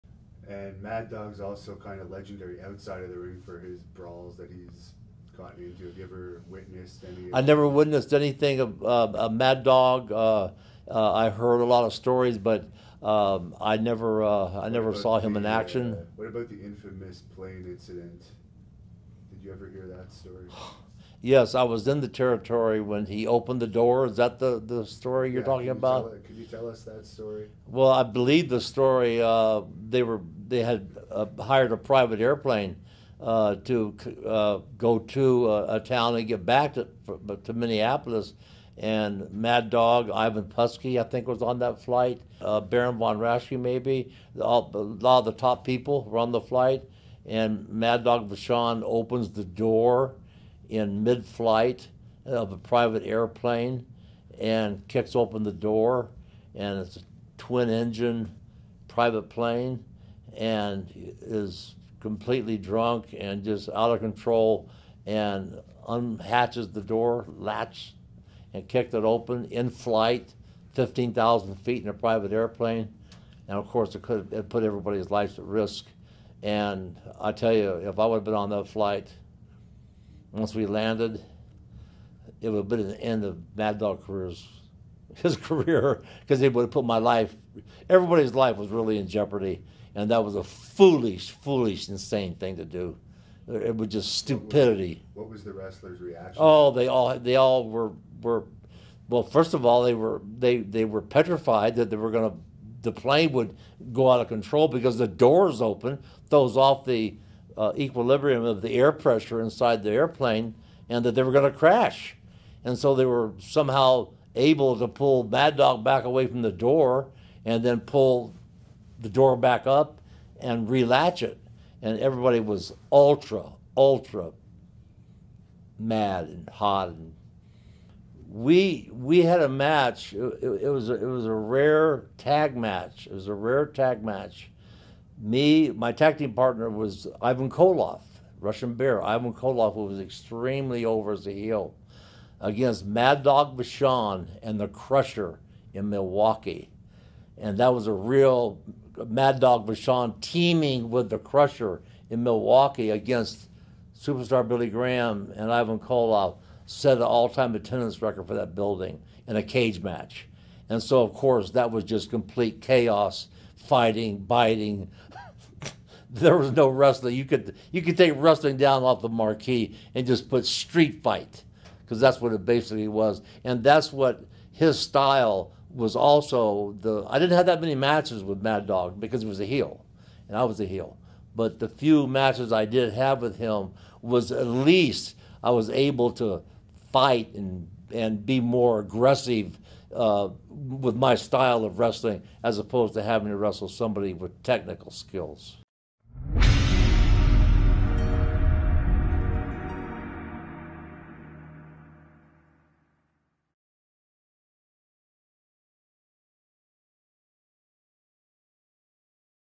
The Untold Story of Billy Graham and Mad Dog Vachon
''SUPERSTAR'' BILLY GRHAM ON MAD DOG VACHON ''AIRPLANE INCIDENT''

World Champion "Superstar" Billy Graham talks about fellow WWE Hall of Famer Maurice "Mad Dog" Vachon as a wrester and also discusses the infamous incident where Vachon opened the door to a plane mid flight while intoxicated, risking the lives of everyone else on the flight.

Follow on Instagram, YouTube, Facebook & Twitter @TheHannibalTV